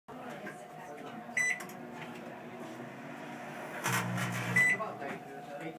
Sparking marshmallow
Burnt marshmallow arcing in the microwave.